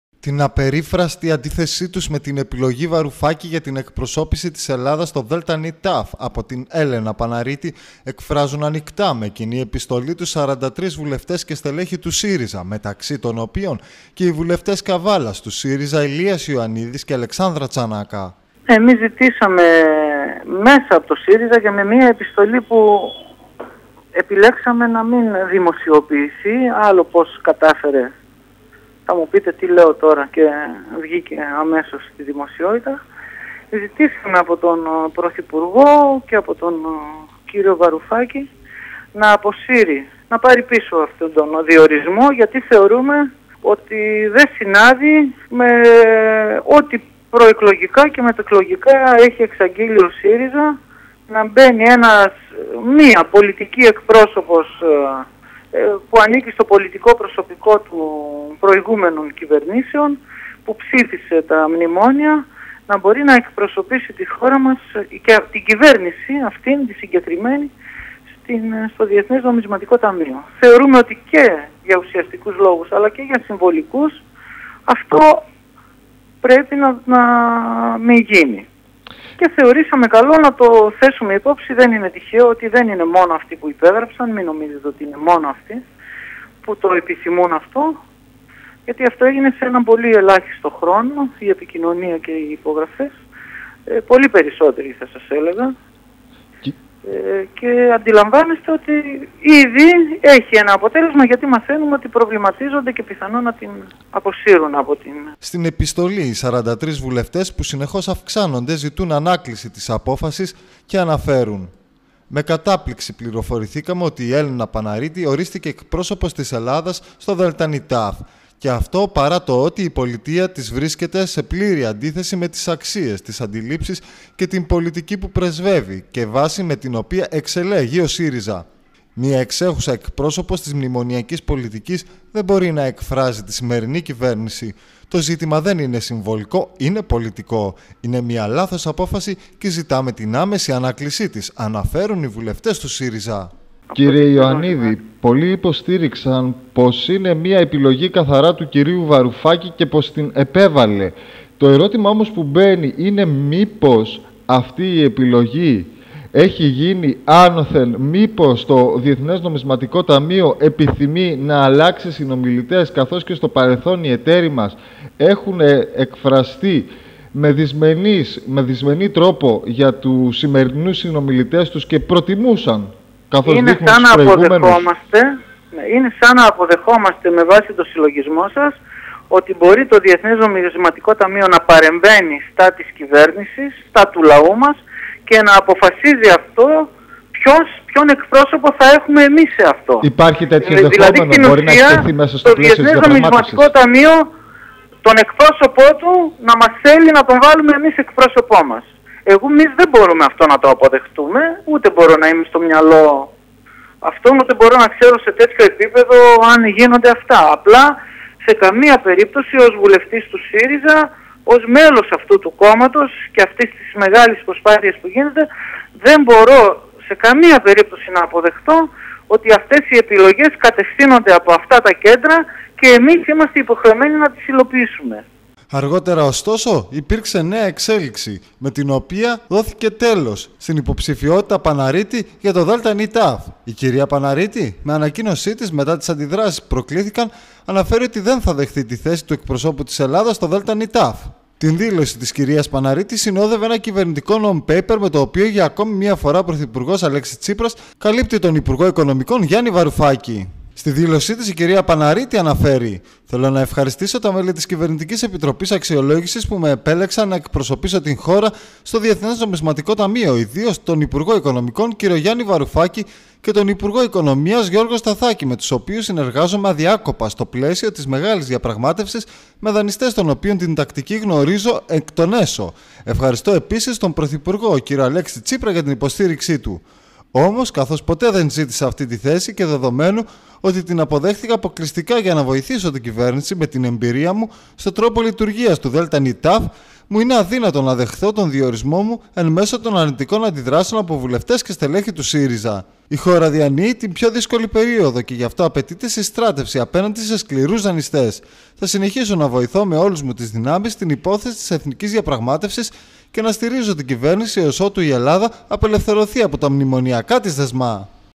Τέλος η Παναρίτη από το ΔΝΤ
Όλη η δήλωση της κας Παναρίτη:

Θέλω να ευχαριστήσω τα μέλη της κυβερνητικής επιτροπής αξιολόγησης που με επέλεξαν να εκπροσωπήσω την χώρα στο Διεθνές Νομισματικό Ταμείο, ιδίως τον Υπουργό Οικονομικών κ. Γιάνη Βαρουφάκη και τον Υπουργό Οικονομίας κ. Γιώργο Σταθάκη με τους οποίους συνεργάζομαι αδιάκοπα στο πλαίσιο της μεγάλης διαπραγμάτευσης με δανειστές των οποίων την τακτική γνωρίζω εκ των έσω. Ευχαριστώ επίσης τον Πρωθυπουργό κ. Αλέξη Τσίπρα για την υποστήριξή του.

Όμως, καθώς ποτέ δεν ζήτησα αυτή την θέση, και δεδομένου ότι την αποδέχθηκα αποκλειστικά για να βοηθήσω την κυβέρνηση με την εμπειρία μου στον τρόπο λειτουργίας του ΔΝΤ [και αντίστοιχων οργανισμών], μου είναι αδύνατον να δεχθώ τον διορισμό μου εν μέσω των αρνητικών αντιδράσεων από βουλευτές και στελέχη του ΣΥΡΙΖΑ.

H χώρα διανύει την πιο δύσκολη περίοδο και για αυτό απαιτείται συστράτευση απέναντι σε σκληρούς δανειστές. Θα συνεχίσω να βοηθώ με όλες μου τις δυνάμεις την υπόθεση της εθνικής διαπραγμάτευσης και να στηρίζω την κυβέρνηση έως ότου η Ελλάδα απελευθερωθεί από τα μνημονιακά της δεσμά.